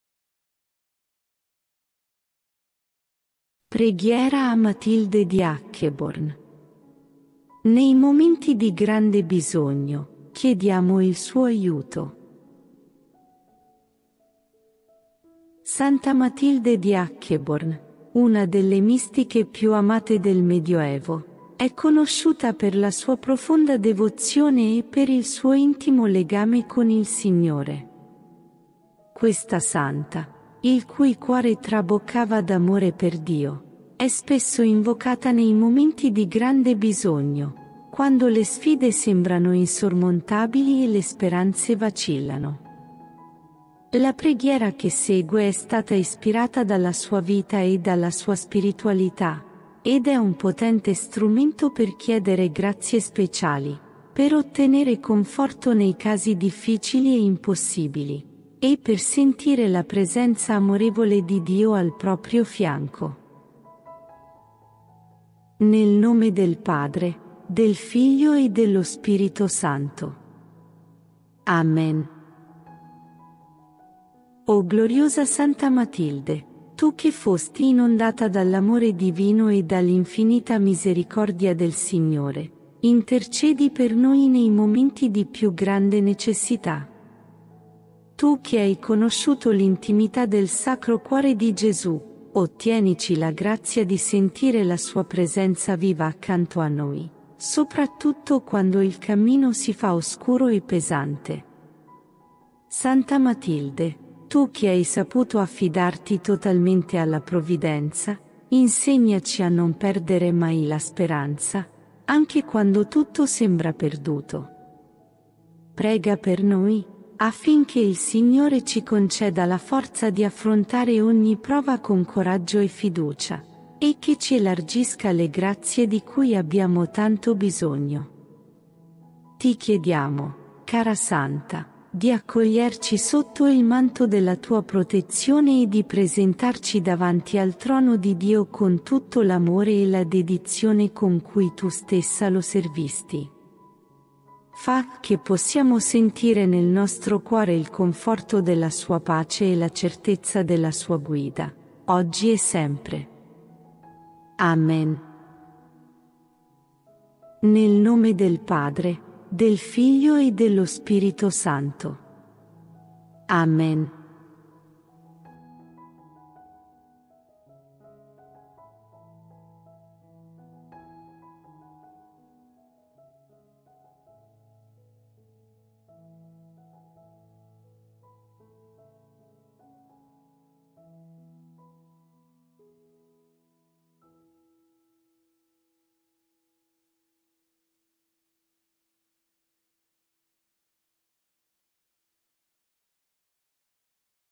Preghiera a Matilde di Hackeborn | Nei Momenti di Grande Bisogno Chiediamo il Suo Aiuto
Santa Matilde di Hackeborn, una delle mistiche più amate del Medioevo, è conosciuta per la sua profonda devozione e per il suo intimo legame con il Signore. Questa santa, il cui cuore traboccava d'amore per Dio, è spesso invocata nei momenti di grande bisogno, quando le sfide sembrano insormontabili e le speranze vacillano. La preghiera che segue è stata ispirata dalla sua vita e dalla sua spiritualità, ed è un potente strumento per chiedere grazie speciali, per ottenere conforto nei casi difficili e impossibili, e per sentire la presenza amorevole di Dio al proprio fianco.